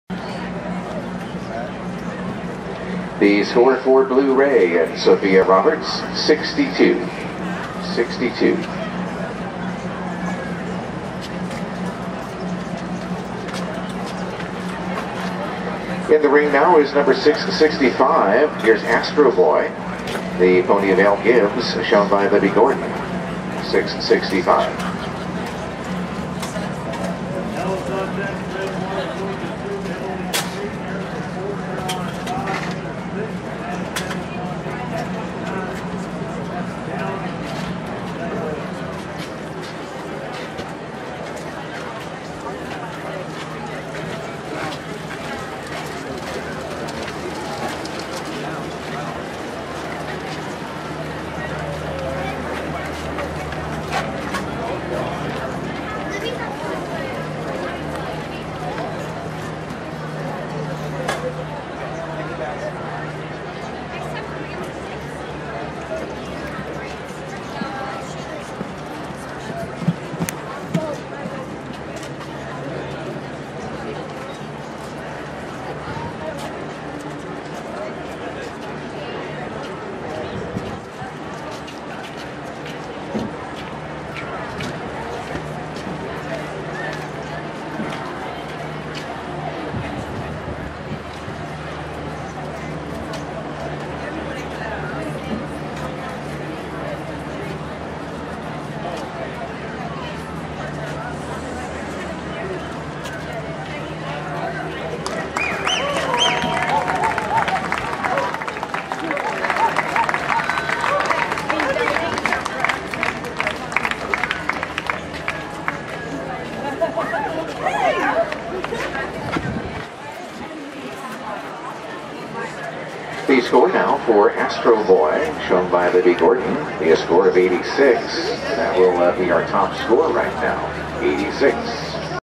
Video of ASTRO BOY ridden by LIBBIE GORDON from ShowNet!
ASTRO BOY ridden by LIBBIE GORDON at Devon Junior Week 2017.  This round is from class 16, $3000 MEDIUM PONY HUNTER STAKE and finished 4 of 29.